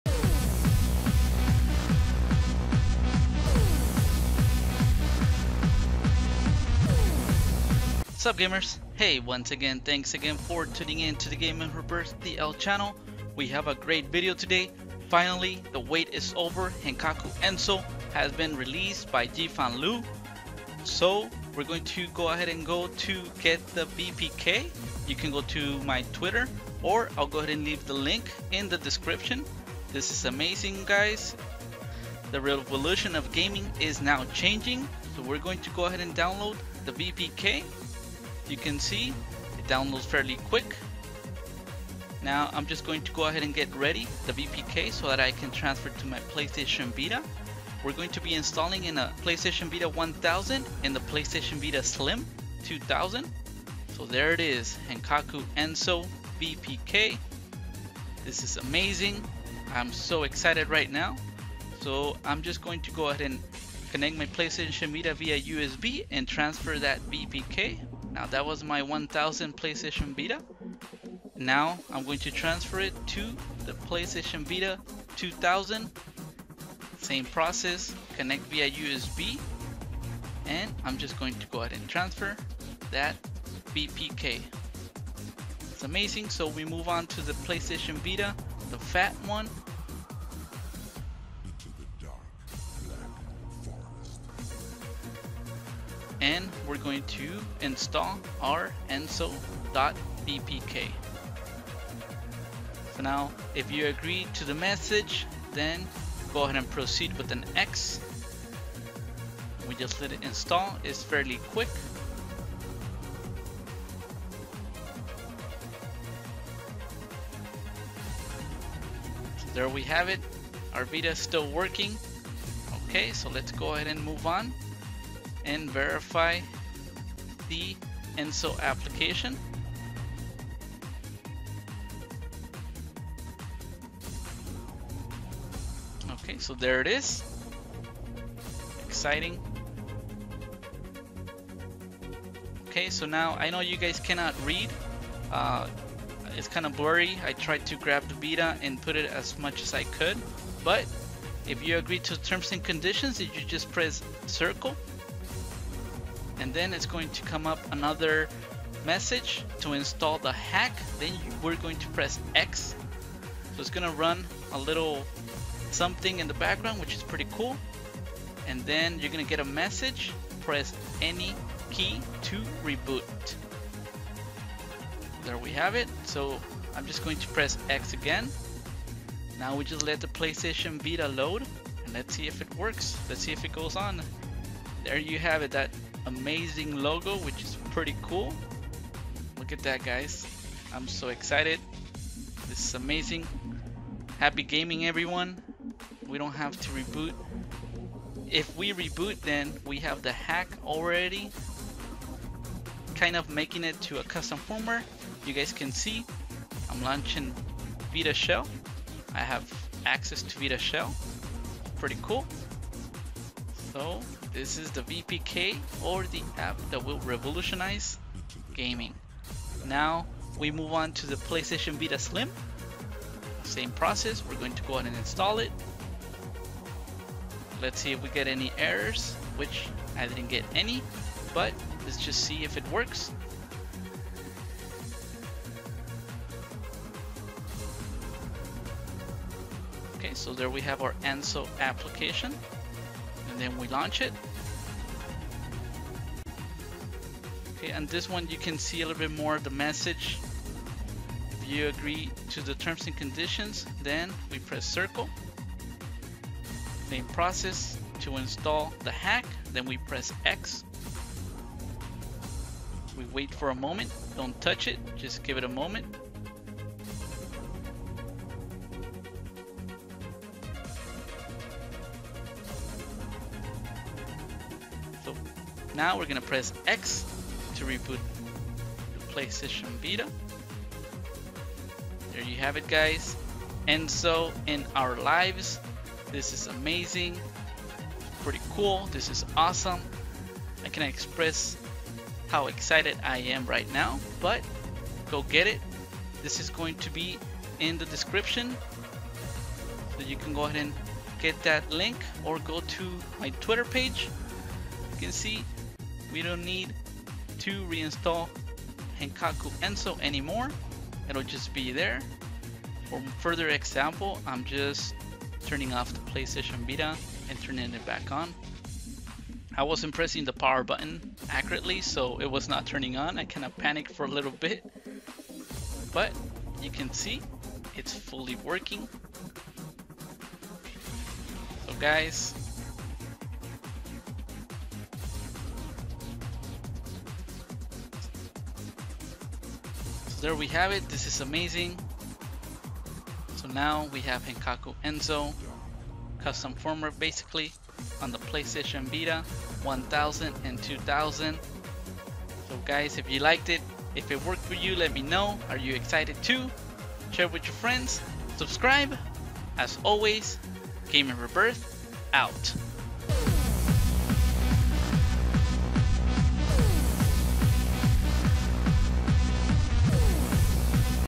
Installing Enso on PS Vita 1000 & PS Vita Slim 2000 3.60! Henkaku PSVita Custom Firmware!
***Works on PS Vita & PS TV!***